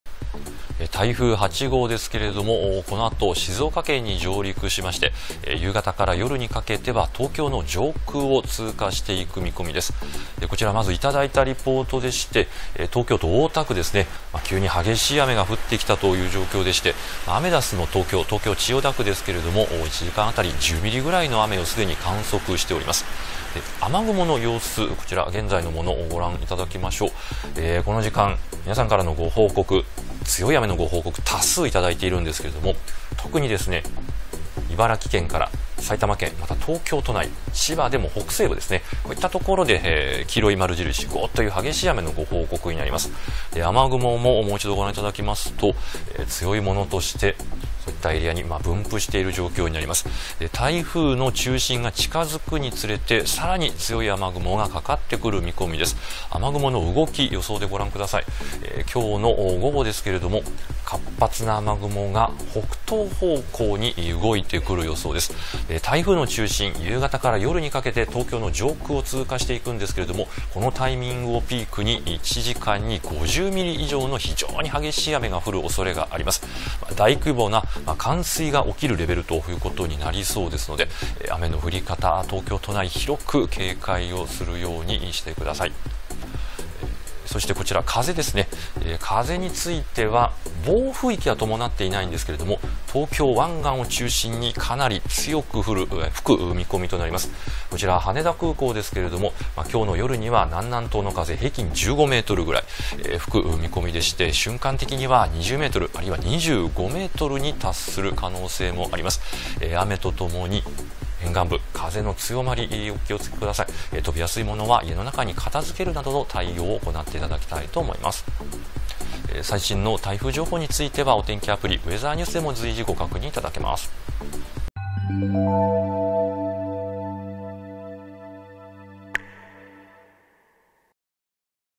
【台風8号】東京直撃、東京への影響は？
今日13日(土)の関東は午前中から局地的に土砂降りの雨になっています。雨は降ったり止んだりを繰り返し、台風が近づく夕方から夜は広範囲で激しい雨となる見込みです。